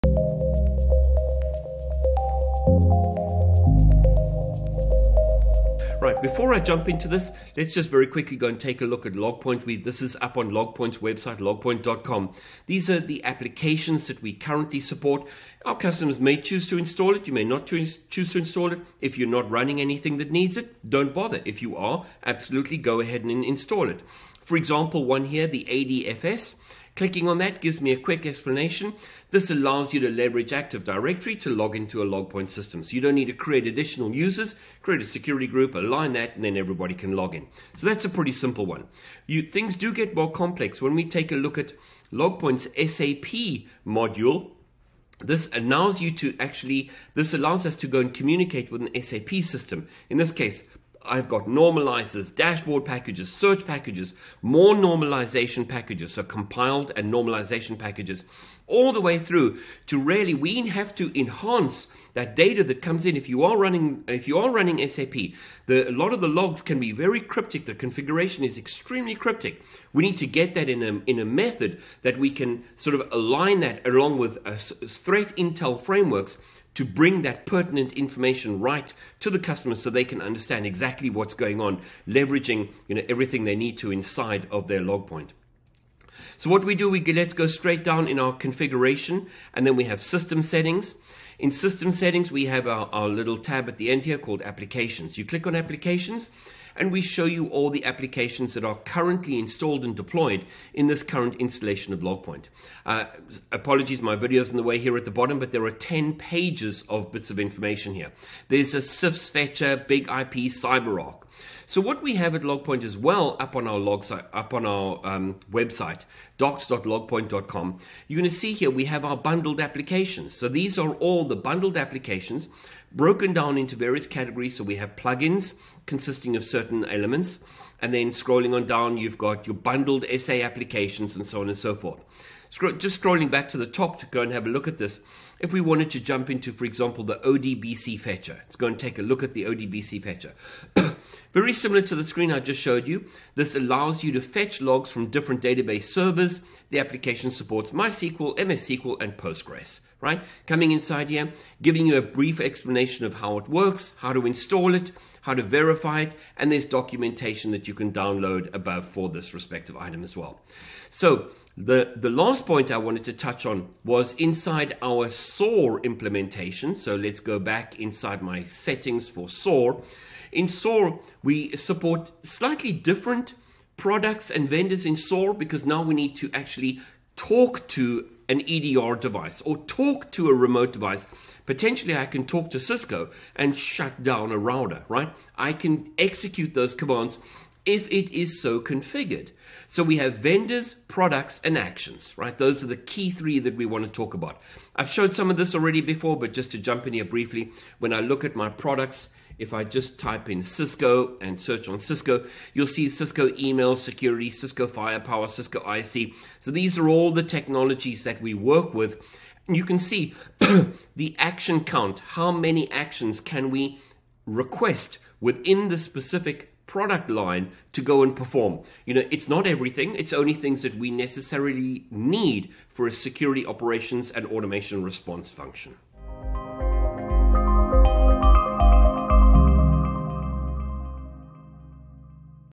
Adding applications to Logpoint
Our senior presales engineer Jason talks all about Logpoint integrations, which add ready-to-use content including normalization, dashboard, searches and reports to support any data source in Logpoint.

TIMESTAMPS
0:00 Details on application that Logpoint support
1:51 Explaining our bundled applications
2:41 Applications on Logpoint’s SOAR implementation